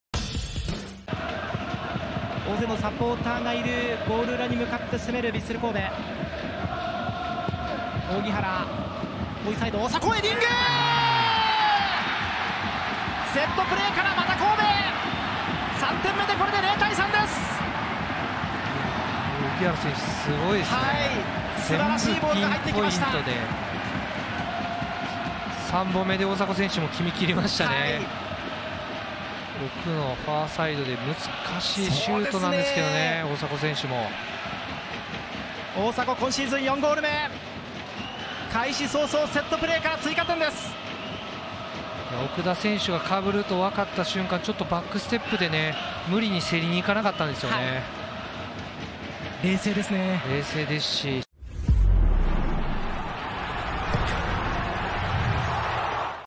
【GOAL】大迫 勇也（49'）｜扇原の右CKをファーサイドでヘディングで叩き込む！｜2024明治安田Ｊ１リーグ 第13節「セレッソ大阪vs.ヴィッセル神戸」
扇原の右CK。インスウィングのボールをファーサイドの大迫がヘディングシュートでねじ込む！神戸が後半開始早々に追加点を決める！

［こちらからの視聴でクラブへ強化費が還元！］